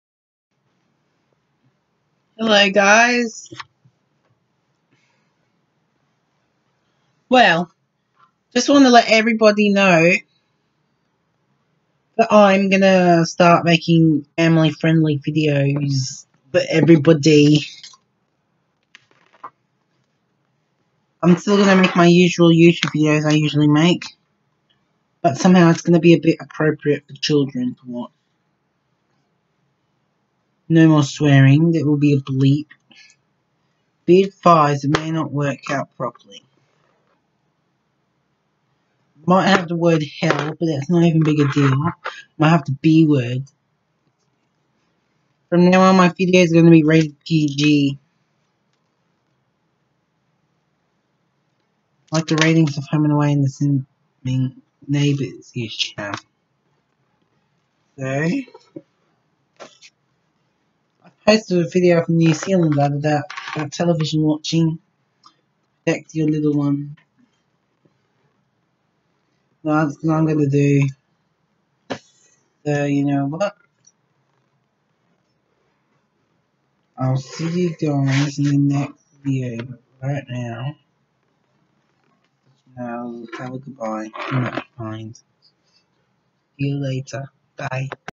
Becoming Family-Friendly
starting from May 15, 2020, this channel will become family friendly rating PG